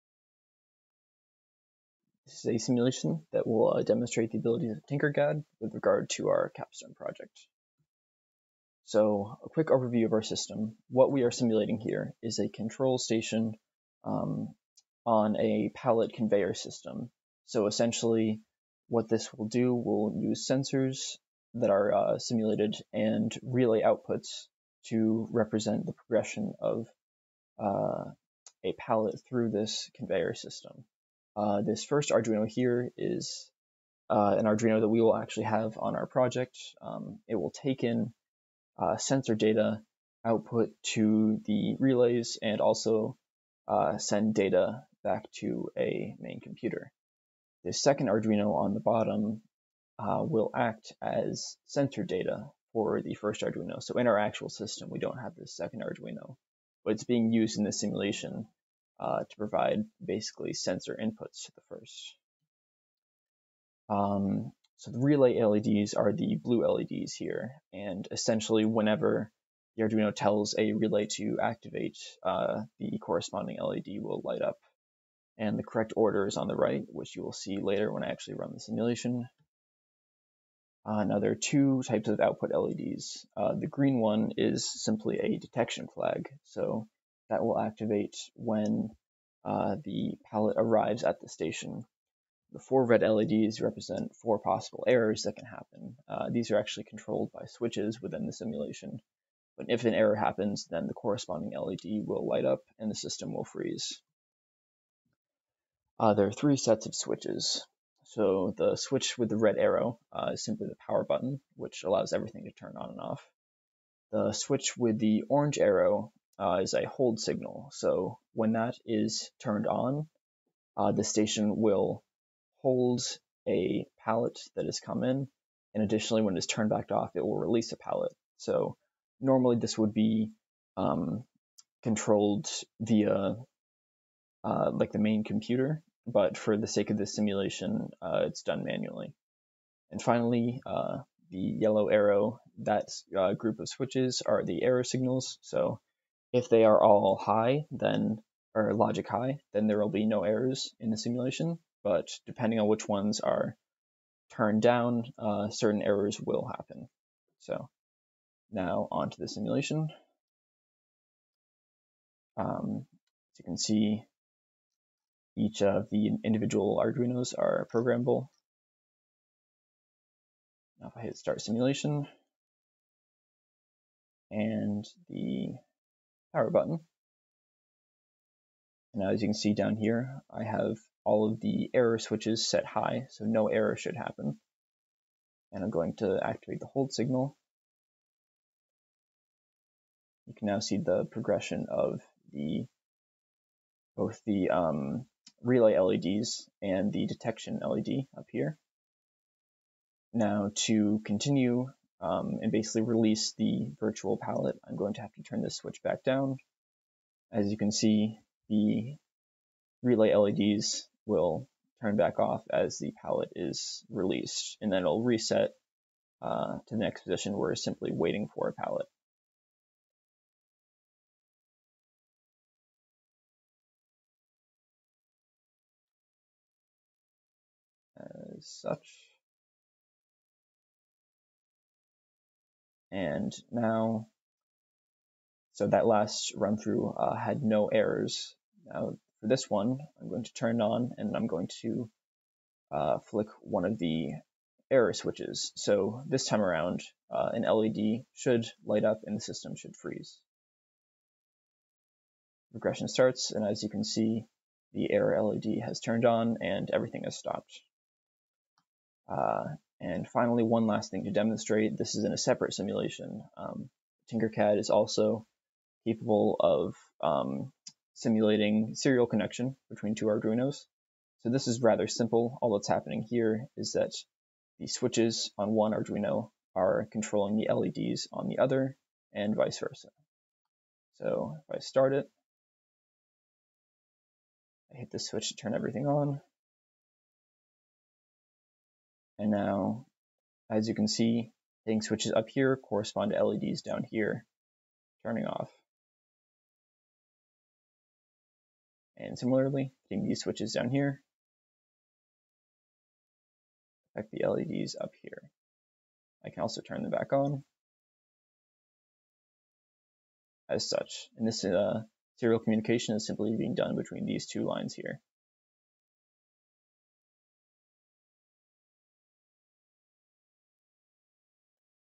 Conveyor Team TinkerCAD simulationvideo v2
Our Spring 2020 Conveyor Upgrade team has been developing a replacement control system for our Manufacturing Innovation Learning Laboratory, also known as the MILL. Outdated hardware has become impossible to replace. The team has conceptualized an Ardunio based replacement. Due to the COVID-19 pandemic, they have been working virtually to propose a design.

Using TinkerCAD they were able to simulate two Ardunos with one representing the conveyor control and the second acting to provide simulation inputs to represent the physical conveyor.